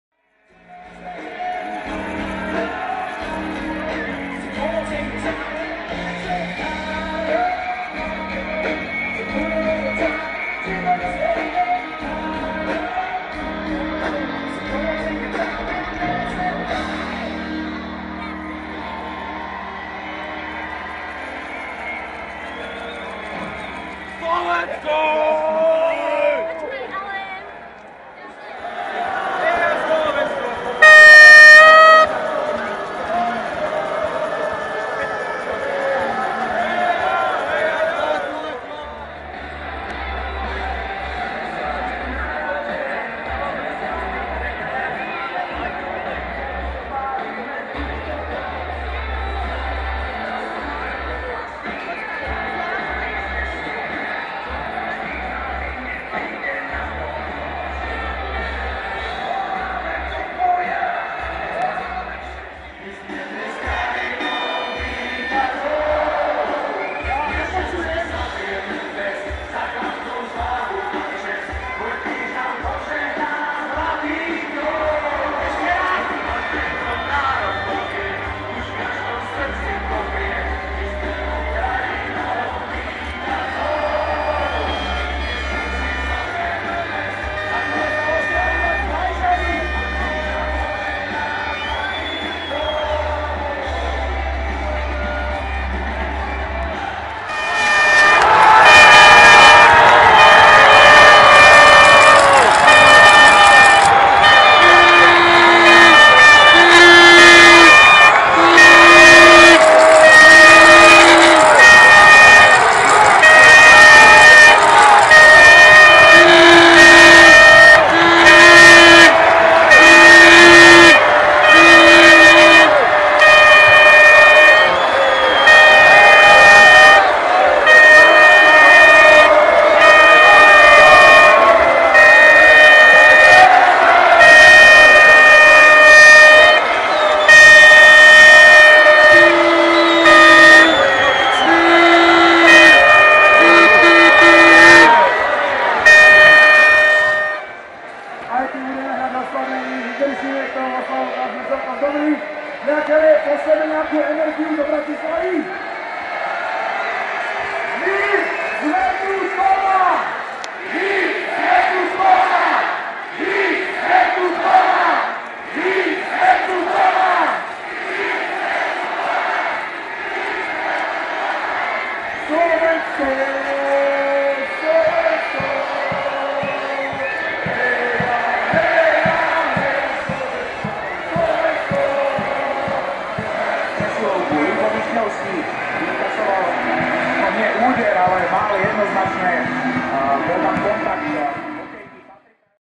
Atmosféra na Amfiteátri Košice - zápas SVK-CZE (časť 1.)
Atmosféra na košickom Amfiteátri počas zápasu Slovensko-Česko (2:3)